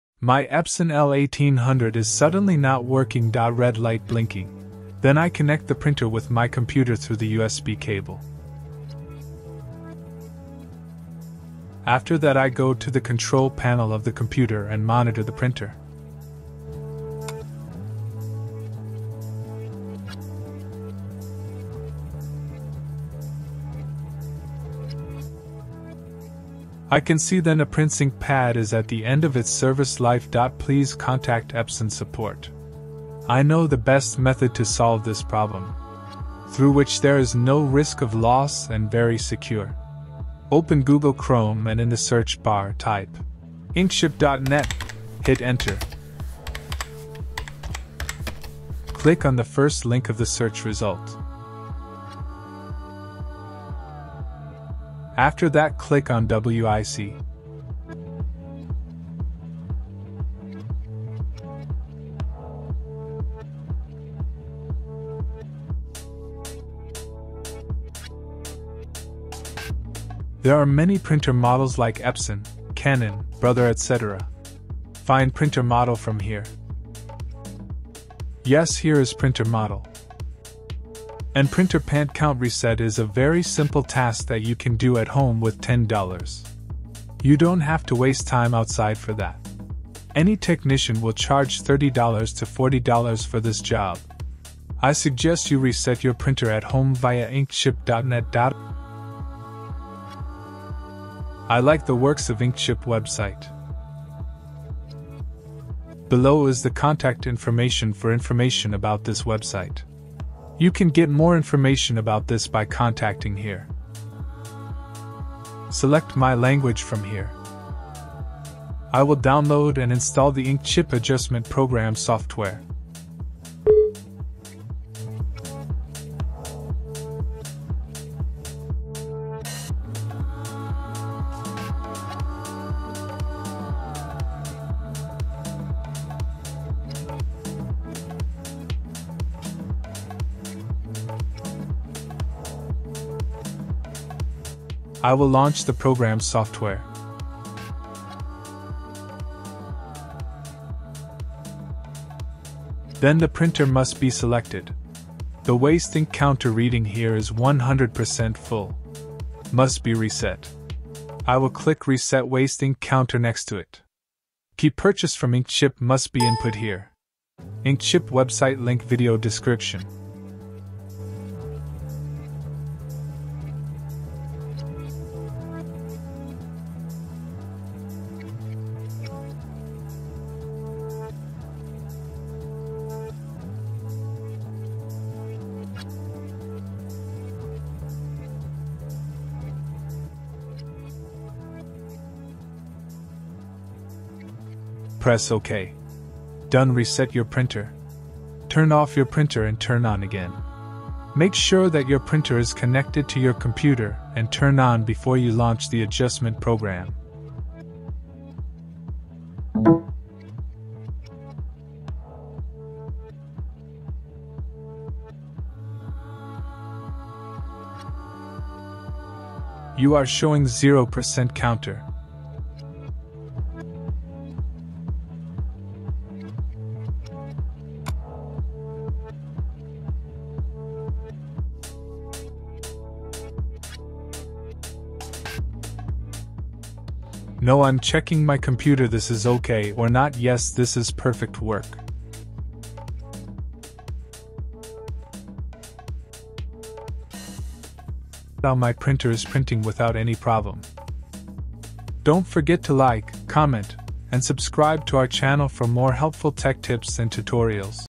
Epson Printer Red Light Blink solution.Reset with adjustment program/Epson L1800 printer||inkchp.net
A printer's ink pad is the end of its service life.Please contact Epson Support.
Epson printer dubbel light blinking.
How to reset epson printer.
In this video, we will guide you on how to resolve the red light blinking issue on your Epson L1800 printer by using the Adjustment Program provided by InkChipnet

Is your Epson L1800 printer showing a red light blinking? Don't worry, we have the solution for you! In this video, we will walk you through the process of resetting your printer with the help of the Adjustment Program from InkChipnet.

First, we will explain what the red light blinking signifies on the Epson L1800 printer and what it means for your printing experience. Then, we'll introduce InkChip.net as a trusted source for the Adjustment Program needed to resolve this issue.

Next, we'll guide you through the steps of downloading and installing the Adjustment Program from InkChip.net. With this program, you can easily reset your Epson L1800 printer and restore it to its normal functionality.

Once the Adjustment Program is installed, we'll demonstrate the exact steps you need to follow to reset your printer successfully. This process will clear any ink-related issues and allow you to continue printing without interruptions.

Furthermore, we'll provide some troubleshooting tips to address any potential challenges you may encounter during the reset process. It's important to proceed with caution, as using third-party software and adjustments may void your printer's warranty.

By the end of this video, you'll have the knowledge and confidence to solve the red light blinking issue on your Epson L1800 printer.